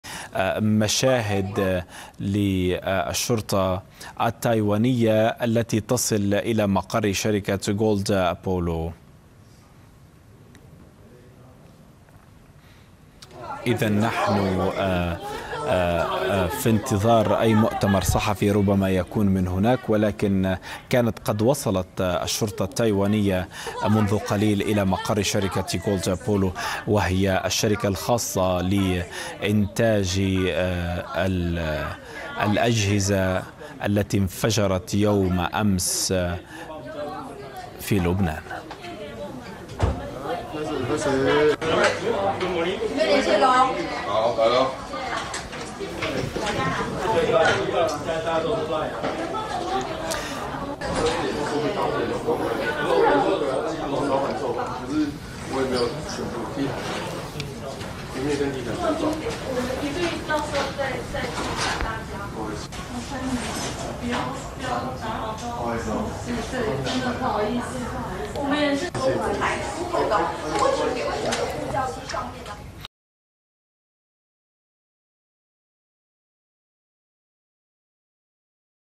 رويترز: الشرطة التايوانية تصل إلى مقر شركة غولد أبولو
مشاهد لوصول الشرطة التايوانية إلى مقر شركة غولد أبولو